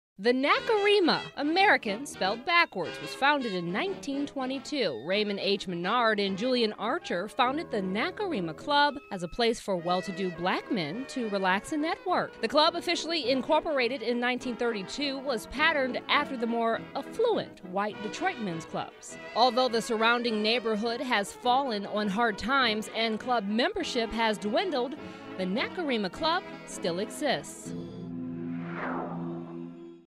Honoring Detroit's Black History: Nacirema Club
In recognition of Black History Month 2020, we've put together a series of short videos to honor and preserve the legacies of some of our most beloved neighbors.